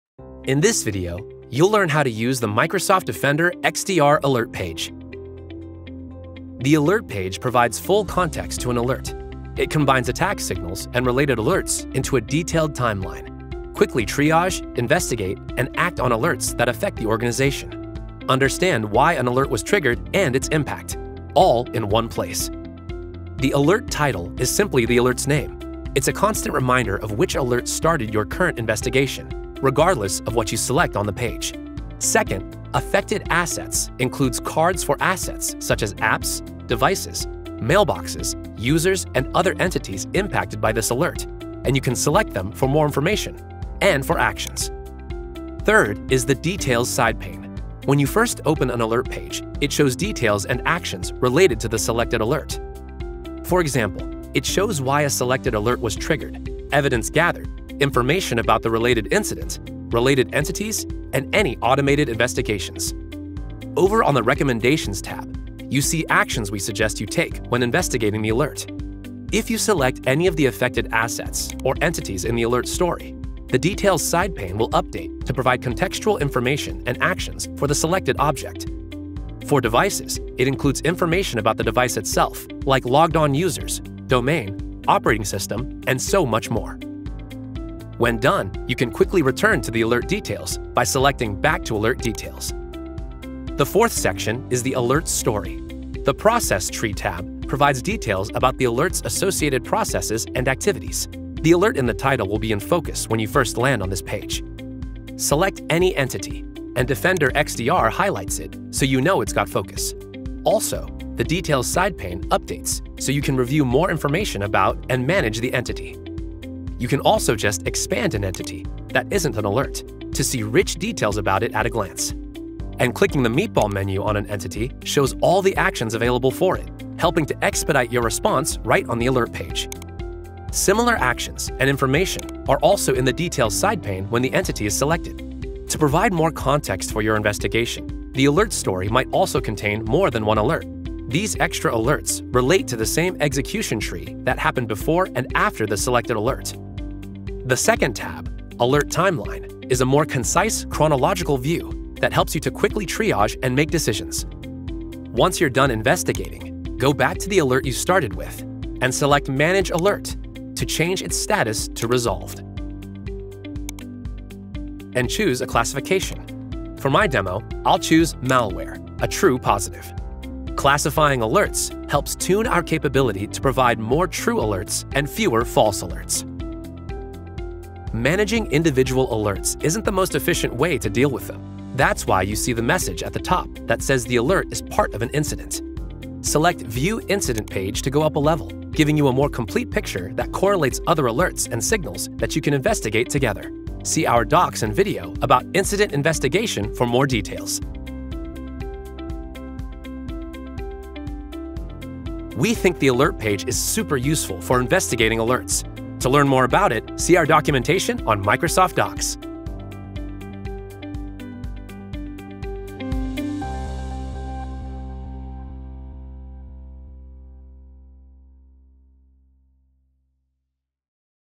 How to use the Alert page - Microsoft Defender XDR
Discover how to navigate the Microsoft Defender XDR alert page - a comprehensive view that pulls together attack signals and related alerts into a detailed timeline to efficiently triage, investigate, and respond to alerts that impact your organization.